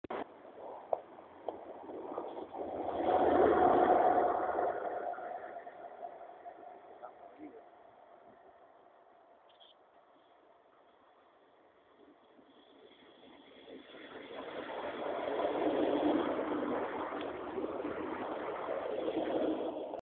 H S T &Arriva Trains at Cockett Bank
Was filmed while waiting for Tornado to arrive H S T and Arriva Train passing each other at Cockett Bank 25th July 20009